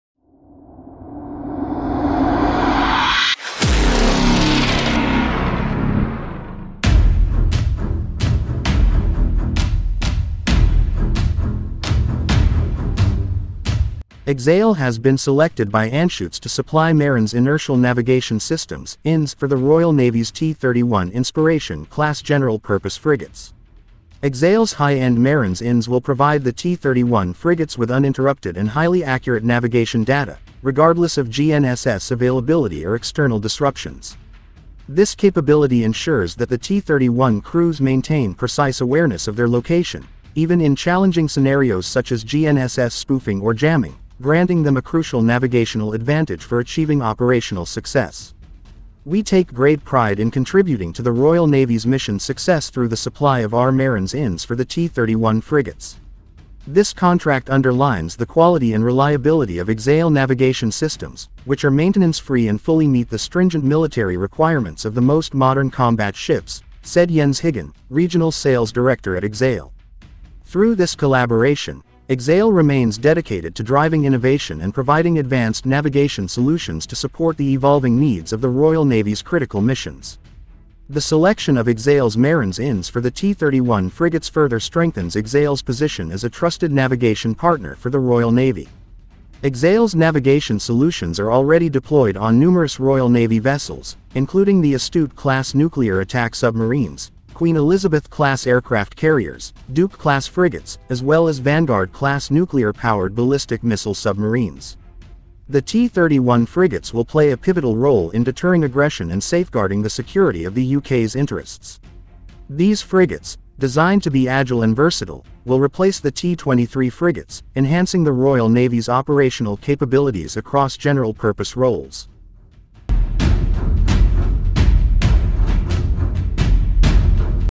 UK T31 Inspiration-class frigates will gets Inertial Navigation System from Exail
Exail has been selected by Anschutz to supply Marins inertial navigation systems (INS) for the Royal Navy’s T31 Inspiration-class general-purpose frigates. Exail’s high-end Marins INS will provide the T31 frigates with uninterrupted and highly accurate navigation data, regardless of GNSS (Global Navigation Satellite System) availability or external disruptions.